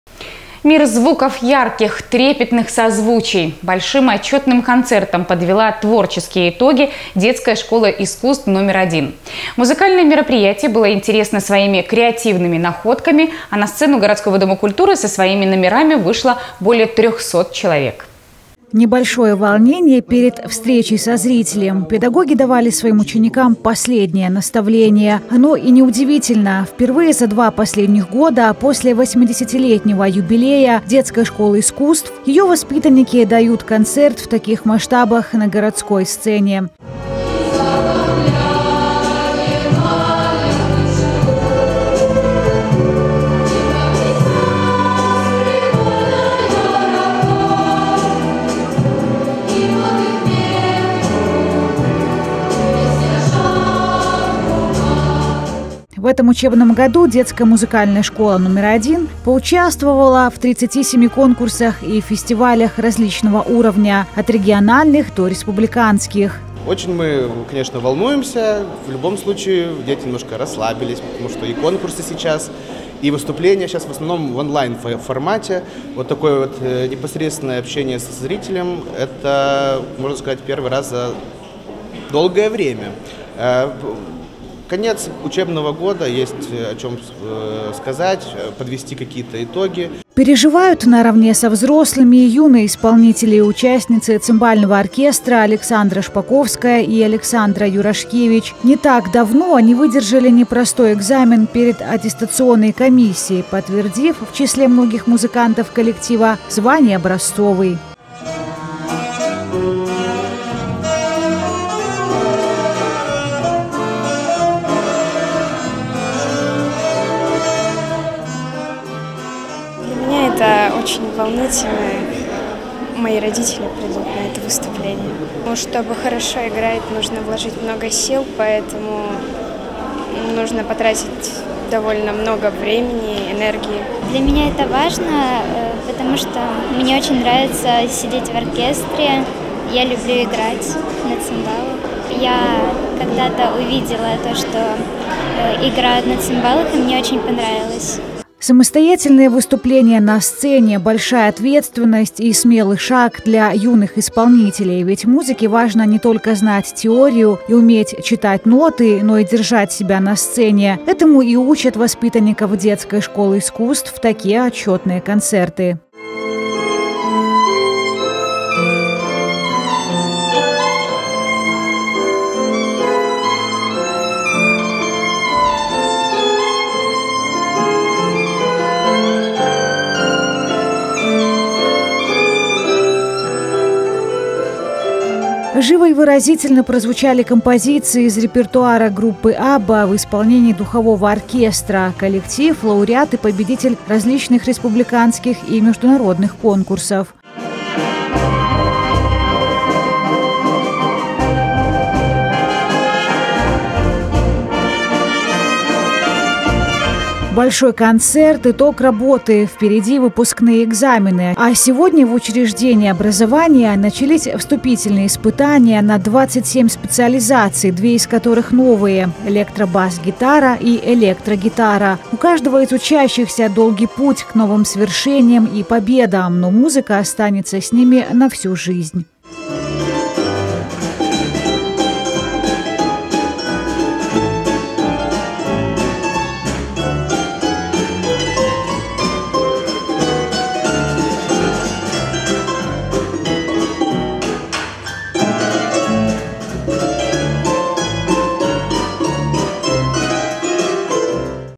Мир звуков ярких, трепетных созвучий. Отчетный концерт детской школы искусств
Мир звуков ярких, трепетных созвучий. Большим отчетным концертом подвела творческие итоги детская школа искусств. Музыкальное мероприятие было интересно своими креативными находками, а на сцену городского Дома культуры со своими номерами вышло более трехсот человек.